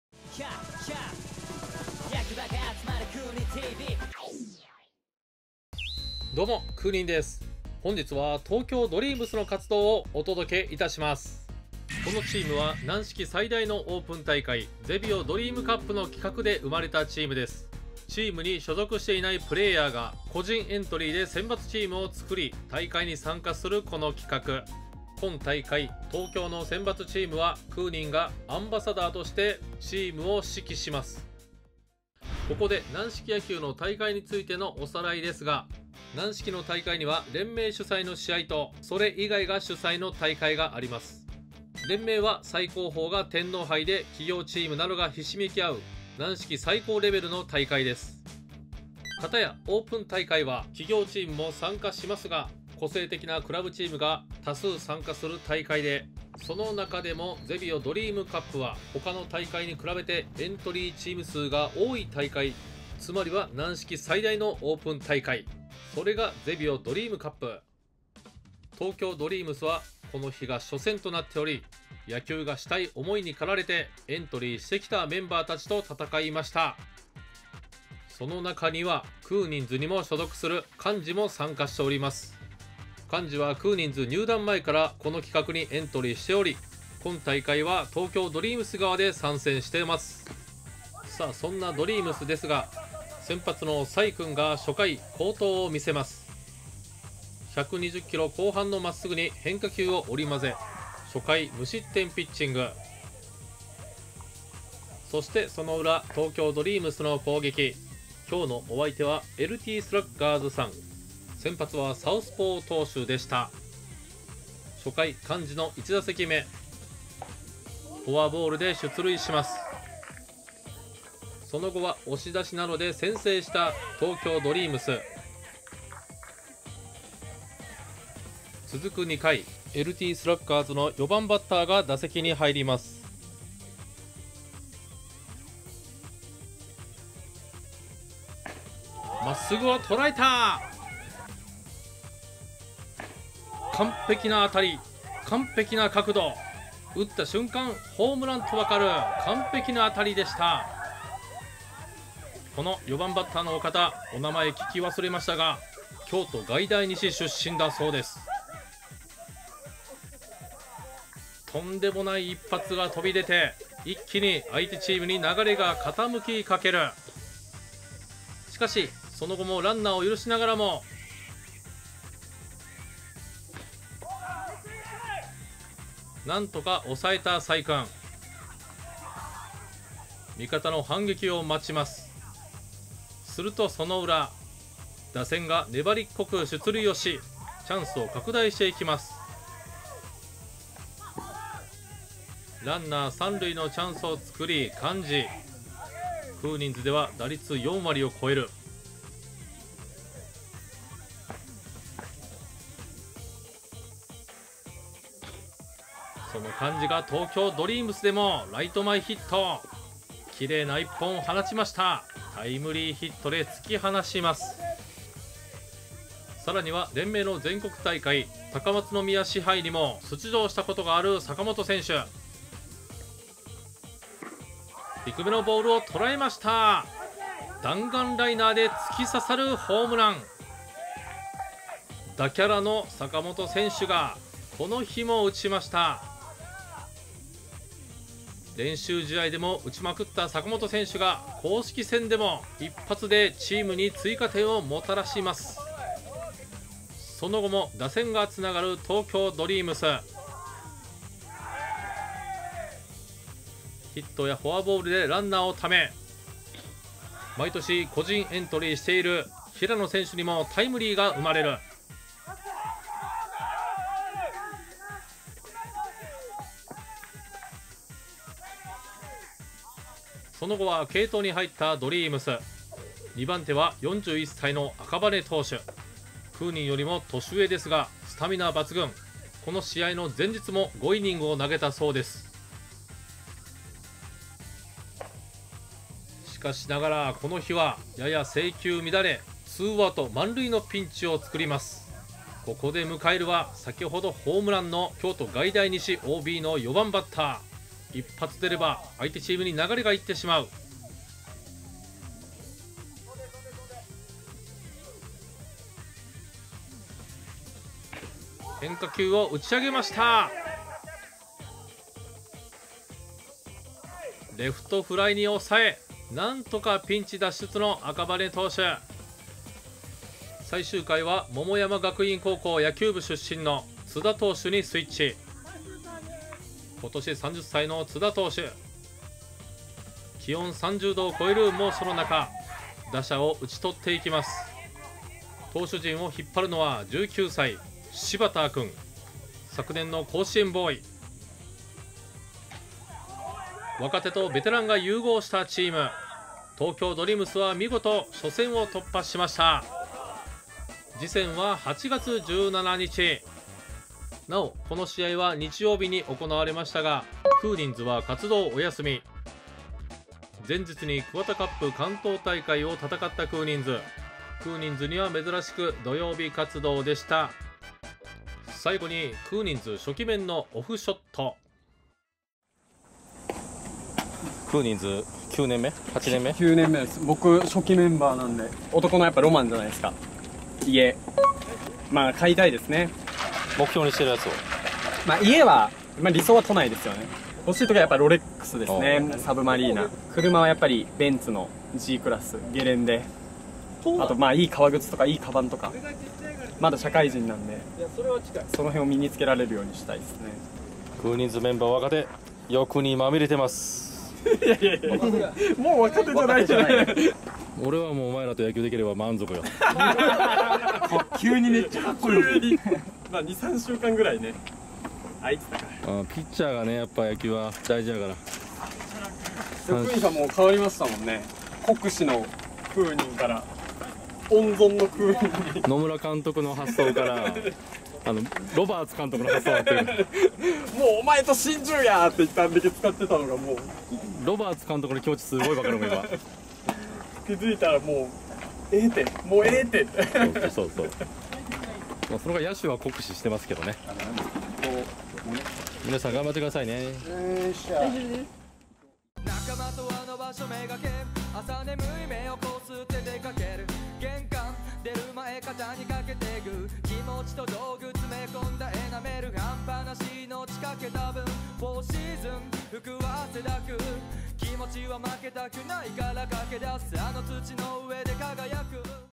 負けたら即解散…クーニン選抜チームの夏開幕！京都外大西の怪物と対決
クーニンズがアンバサダーを務める選抜チーム・東京ドリームス初戦！
負ければそこで解散！